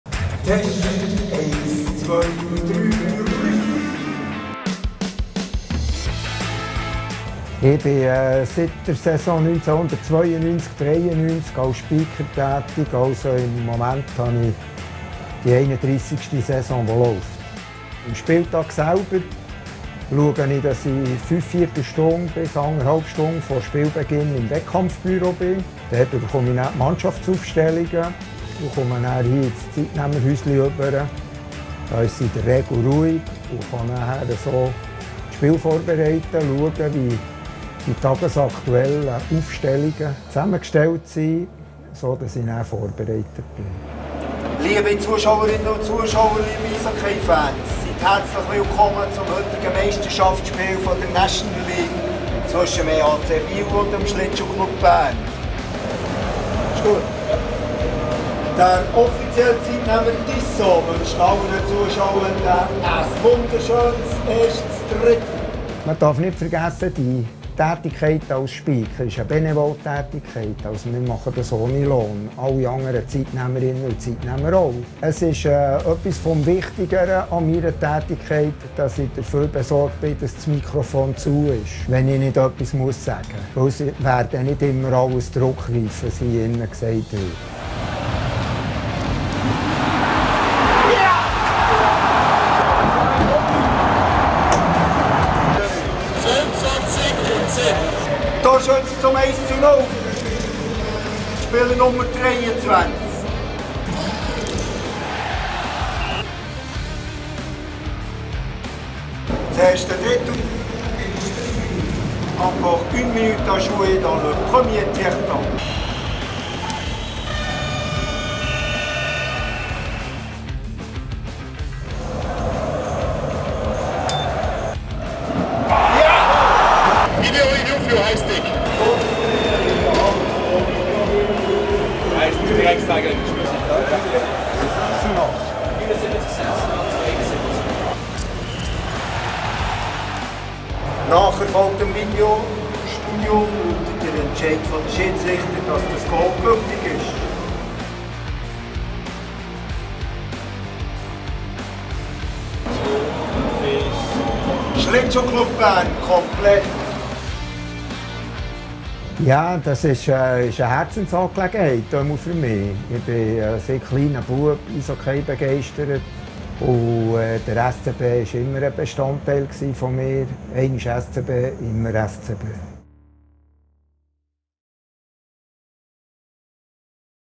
SCB | «Eisblicke» mit SCB-Speaker | Gerhard Ryser
In dieser Ausgabe der SCB-Serie «Eisblicke» begleiten wir an einem Matchtag den langjährigen SCB-Speaker Gerhard Ryser. Schau rein und erfahre hautnah, wie sich ein SCB-Spiel direkt vor ihm abspielt.